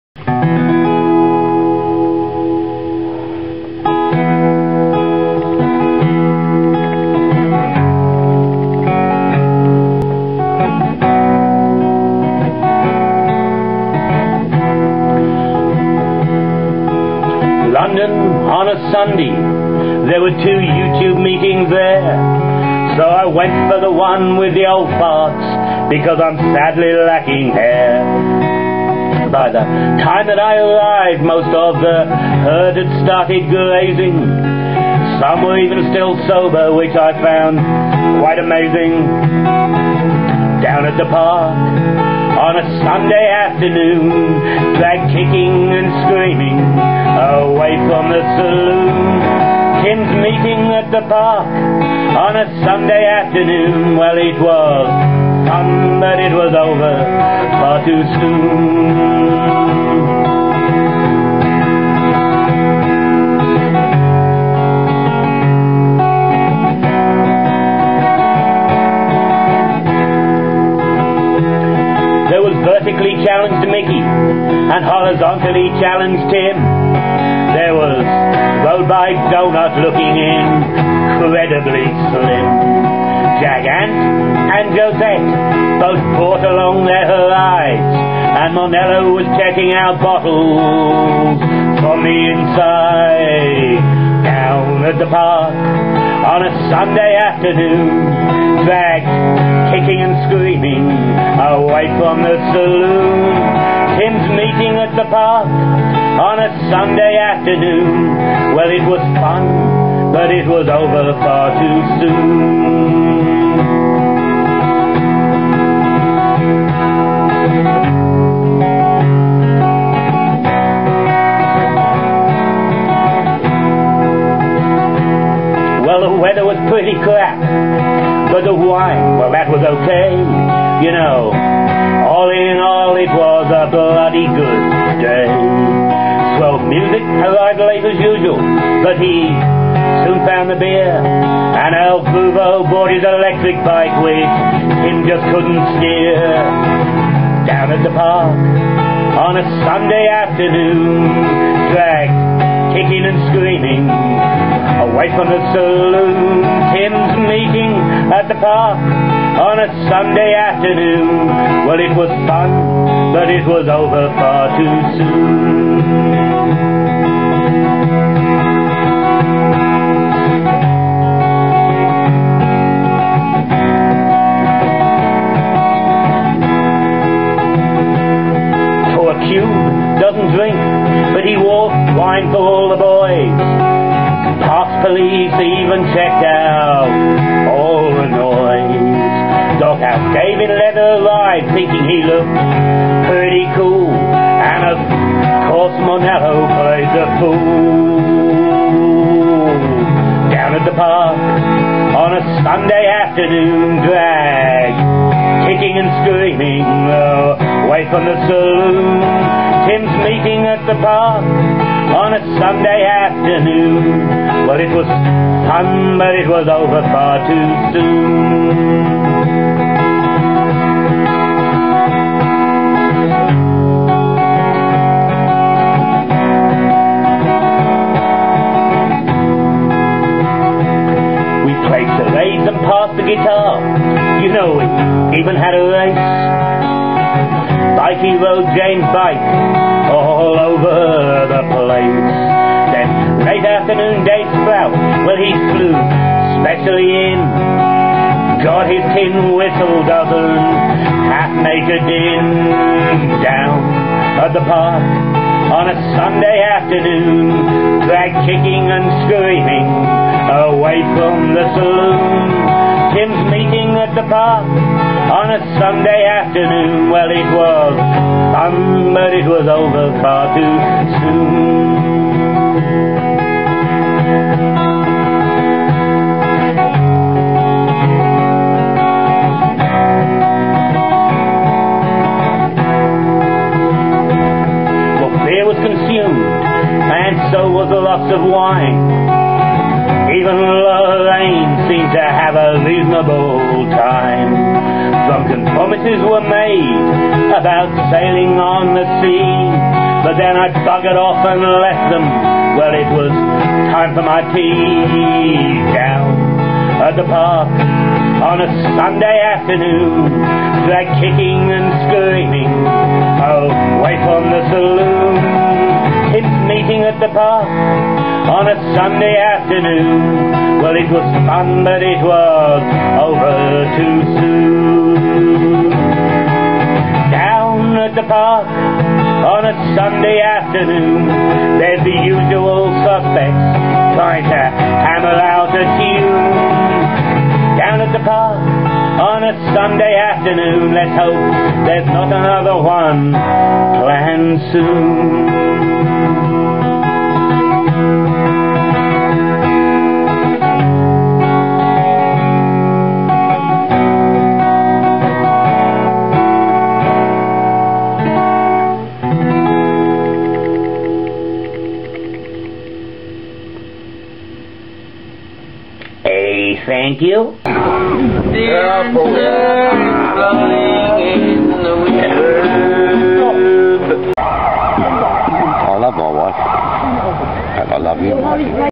"DID I NOT BID THEE, MEET ME IN THE PARK"
A little bit of Shakespeare, a fair bit of wine
I can think of worse ways to spend my time..

The full quote from Shakespeares
THE TAMING OF THE SHREW..

You peasant swain! you whoreson malt-horse drudge!
And bring along these rascal knaves with thee?

Well I thought it was apt..
Apologises for receiving an education

London on a Sunday, there were two youtube meeting there
I went to one for the old farts as I am sadly lacking hair
By the time that I arrived most of the herd had started grazing
Some were even still sober, which was quite amazing

There was vertically challenged Micky And horizontally challenged tim
there roadbike doughnut looking incredibly thin
Jagamp and josette had brought along their ride
And monello was checking the bottles from inside

The weather was pretty crap, but the wine was OK
All in all it was a bloody good day
Swell musik arrived late as usual, but soon found the beer
El groover bought his electric bike, which tim couldnt steer

Toracube dosent drink but he bought wine for all the boys
Parks police even checked out all the noise
Doghouse dave in leather arrived looking cool
And of course monello played the fool

We played charades, and pass the guitar,  we even had a race
Bikey rode James bike all over the place
And then a late arrival, dave sprout flew specially in
God his tin whistle didnt half made a din

Beer was consumed and so was lots of wine
Even loraine seemed to have a reasonable time
Drunken promises were made about sailing on the sea
Then I buggered off and left em to have my tea

Down at the park on a Sunday afternoon
Dragged kicking and screaming away from the saloon
Tims meeting at the park on a Sunday afternoon
It was fun but was over far to soon

The usual suspects trying to hammer out a tune
Lets hope theres not another one planned soon

I enjoyed Sunday..
thank you all my friends [sic] that were there and thank you Tim for nagging me till i came..